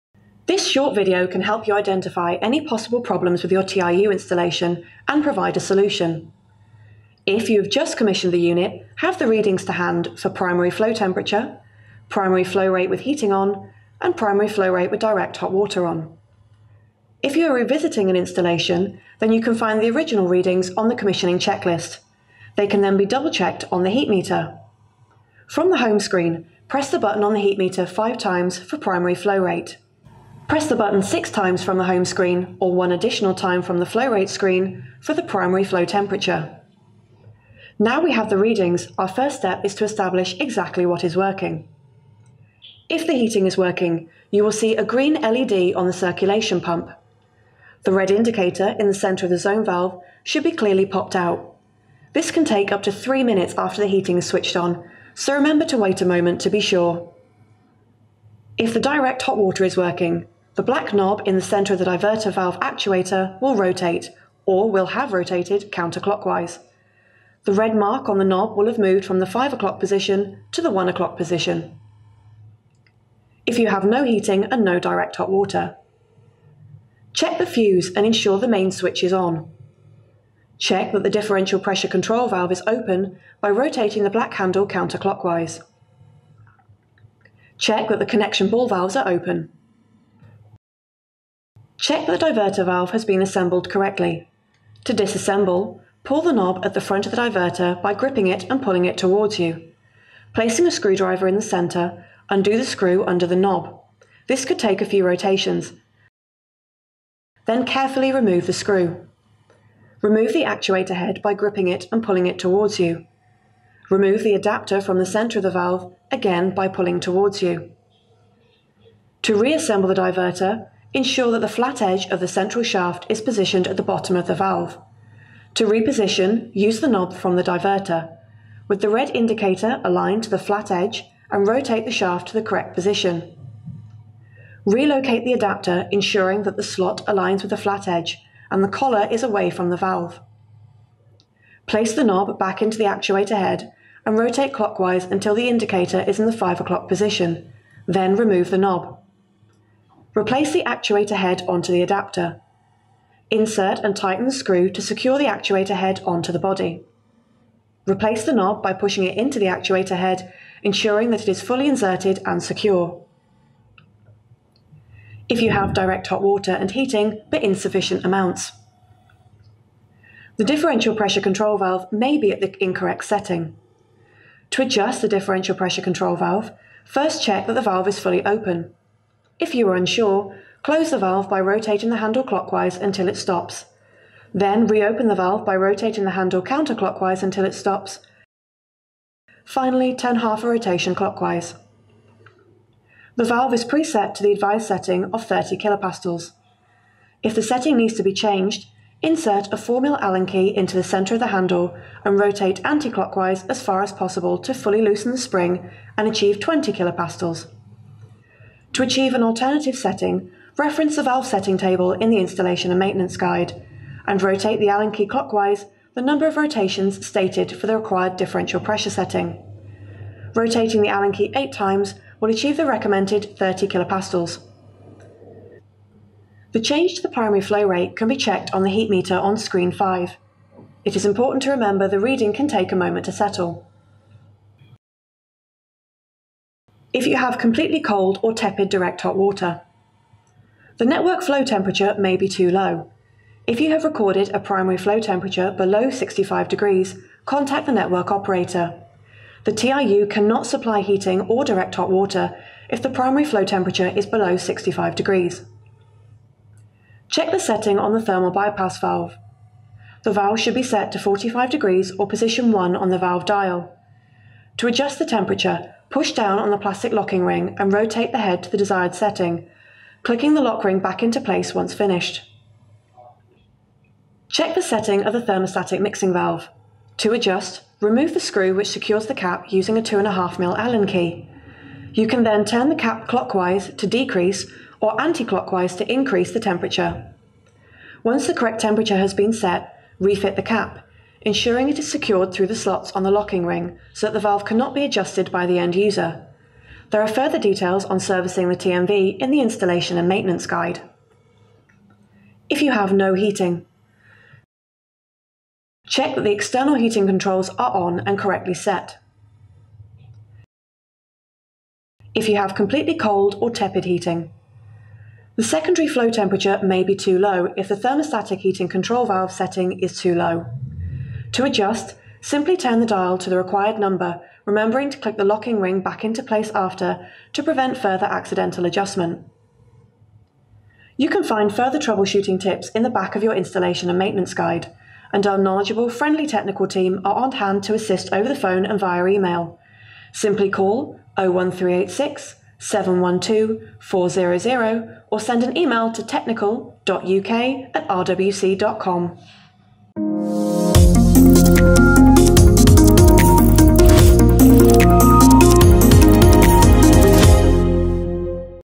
How To Troubleshoot A Radiance TIU - Helping Hand - Reliance UK
Reliance Radiance Thermal Interface Units (TIUs), offer the perfect solution when an instantaneous or stored hot water system is required in a community heating network installation as they can be adapted to exact requirements.

Designed with simplicity and ease in mind, we have put together this short video to help installers see how Reliance Radiance TIUs are troubleshooted and how to solve common problems.